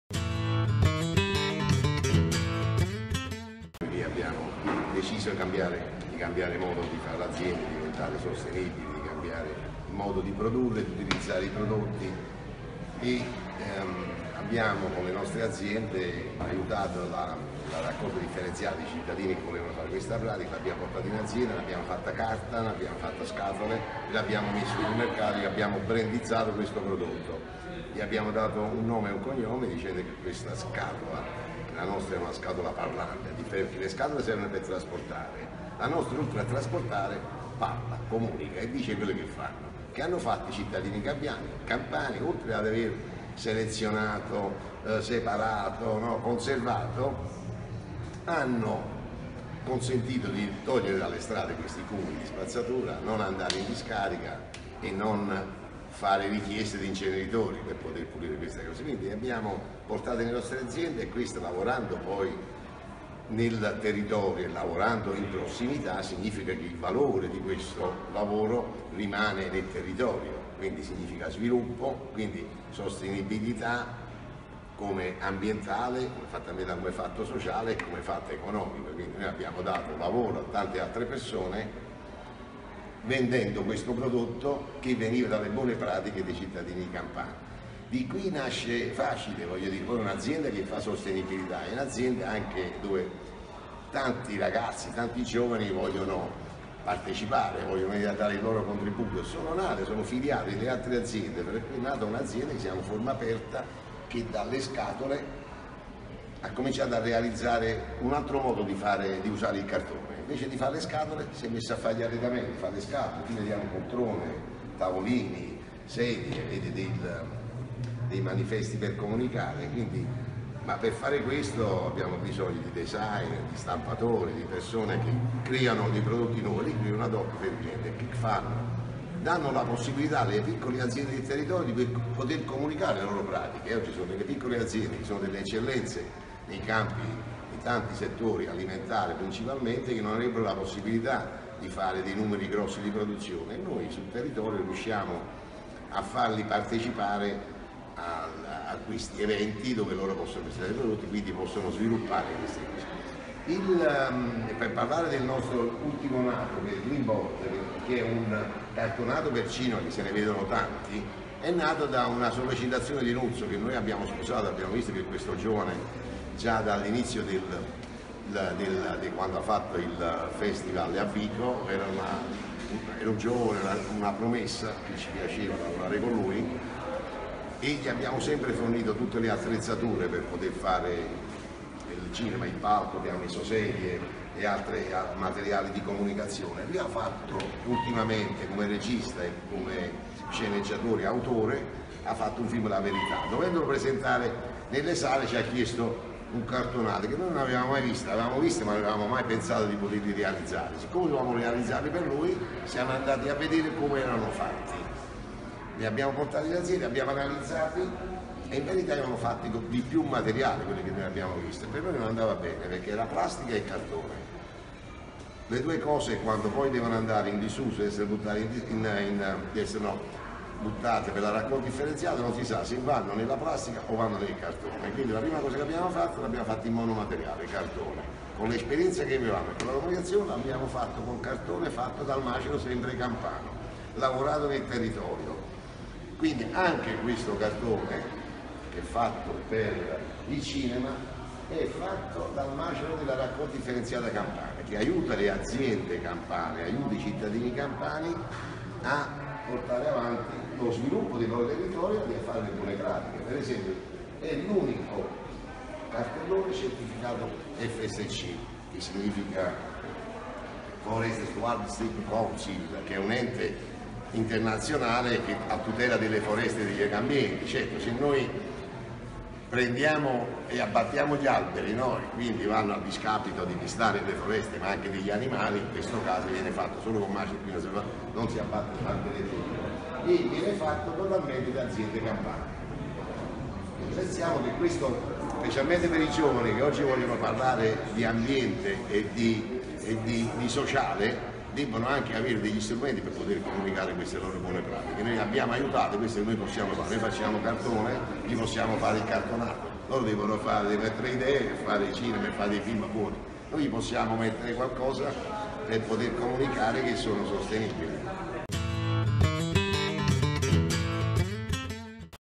Intervista Aldo Savarese amministratore unico Sabox
Aldo Savarese, amministratore unico Sabox, ha presentato in occasione del Social World Film Festival un prodotto rivoluzionario: Greenboard, primo pannello cinematografico interamente realizzato con il macero dei rifiuti campani